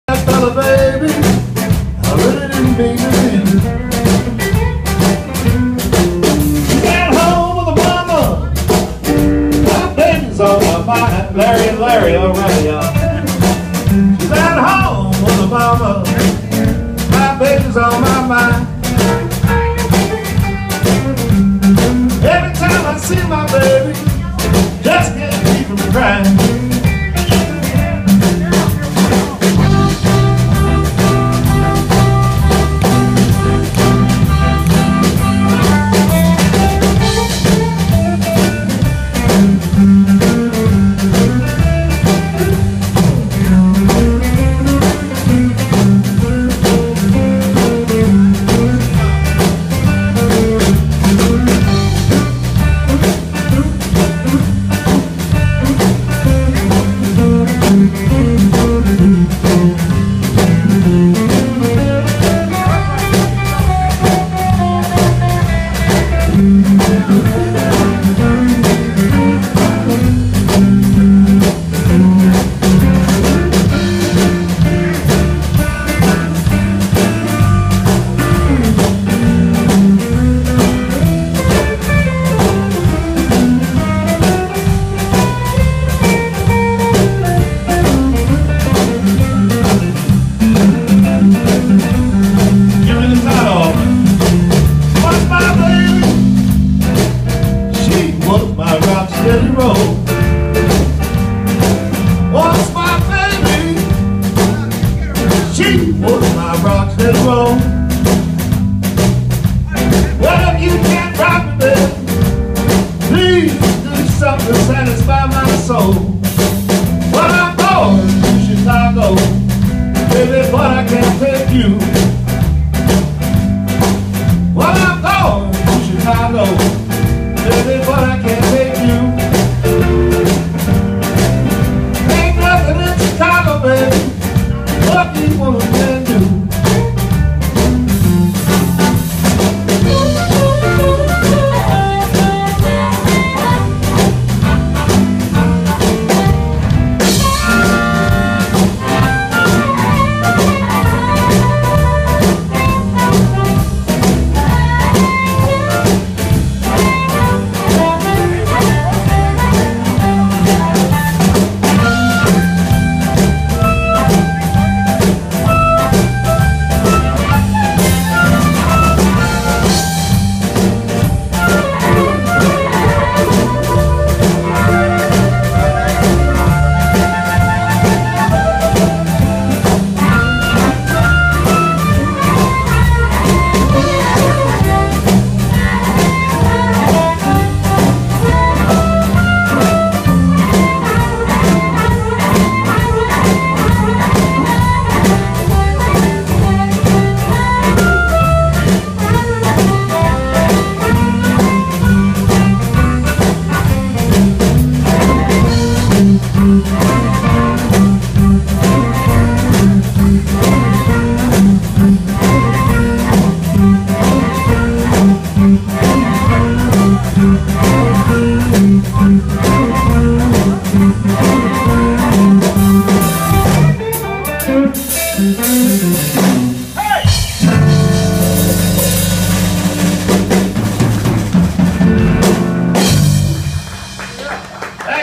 Louisville Fats and the Rhythm Cats at Stevie Rays Blues Bar in Louisville ky
Louisville Fats and the Rhythm Cats. wed. the 15th 2014